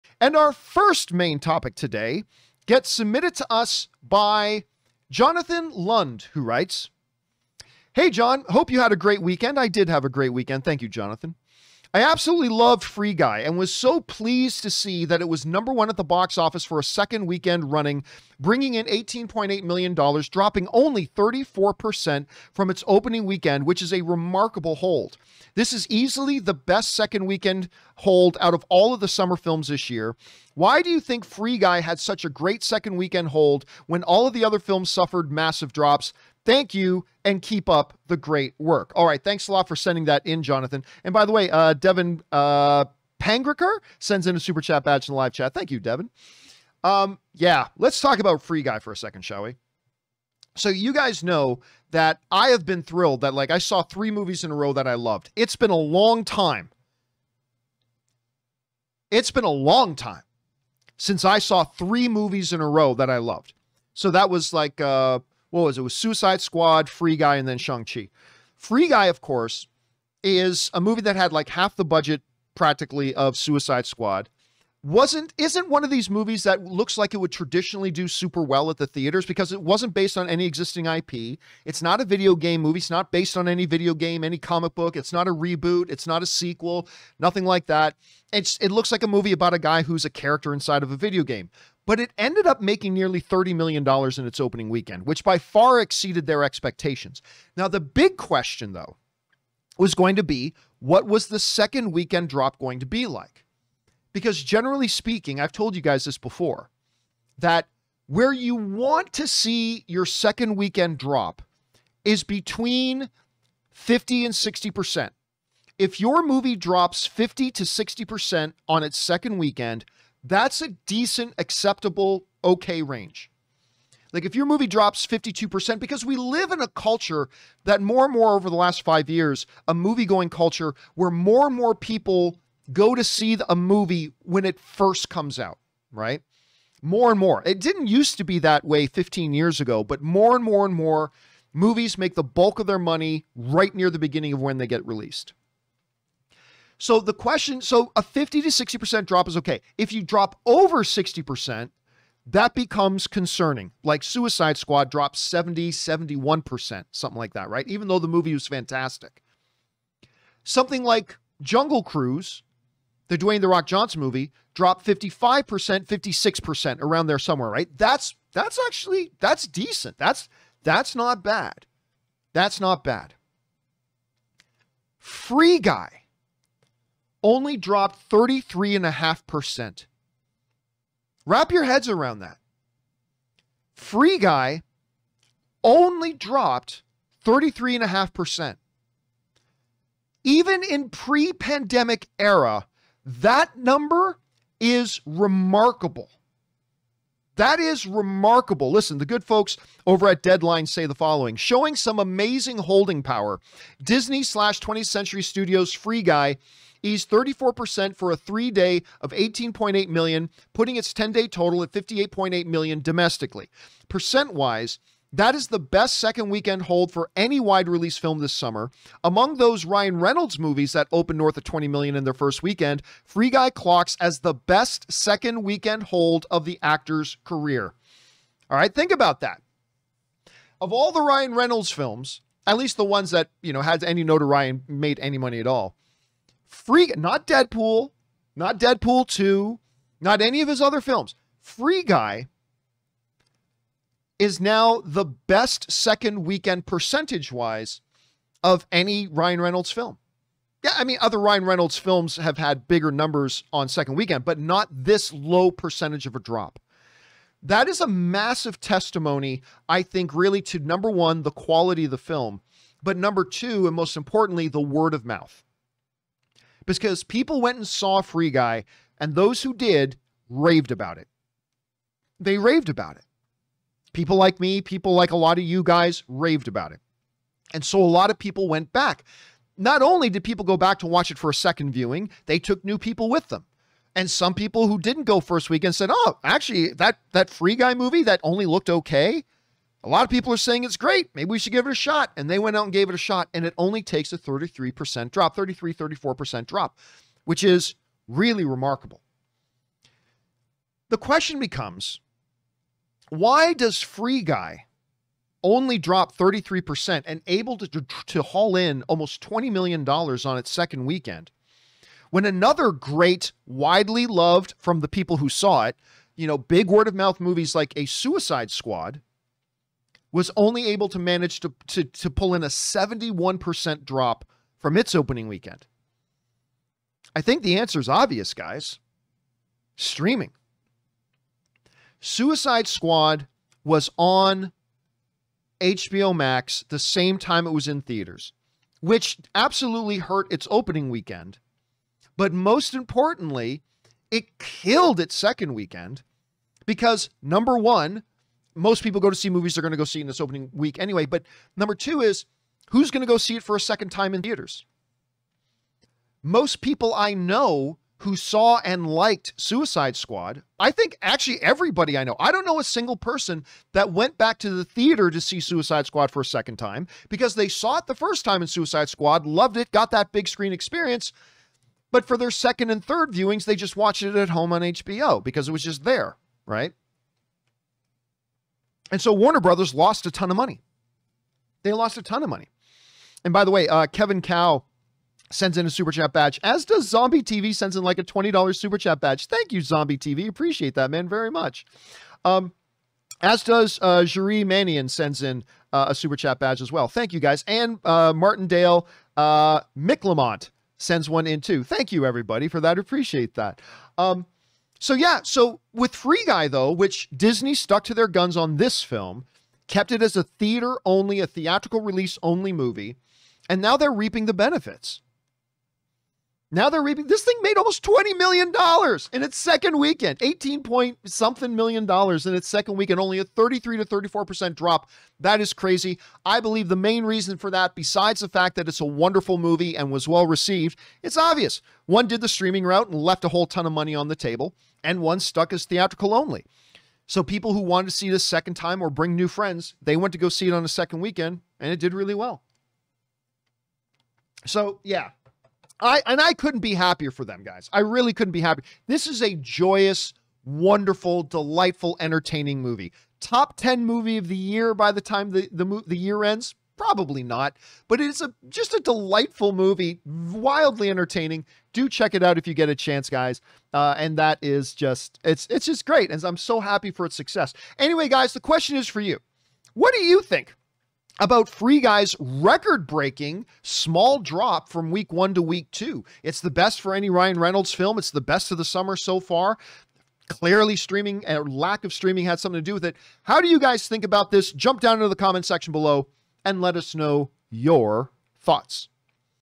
Free Guy Holds Seriously Strong At Box Office
Viewer Question:
Jonathan Lund - Hi John I hope you had a great weekend. I absolutely loved Free Guy and I was so pleased to see that it was number 1 at the box office for the second weekend running bringing in $18.8 million dropping only 34% from is opening weekend which is a remarkable hold. This is easily the best second weekend hold out of all the summer films this year. Why do you think Free Guy had such a great second weekend hold when all the other films suffered massive drops? Thank you and keep up the great work!

Mic pre-amps

Streaming Software
Wirecast Pro